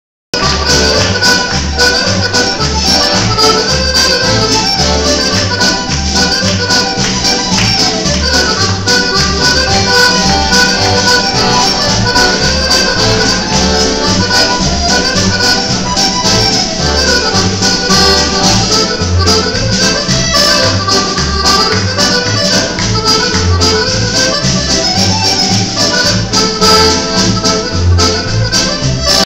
Scottish Caleigh
Our delegation was special guests at the Caleigh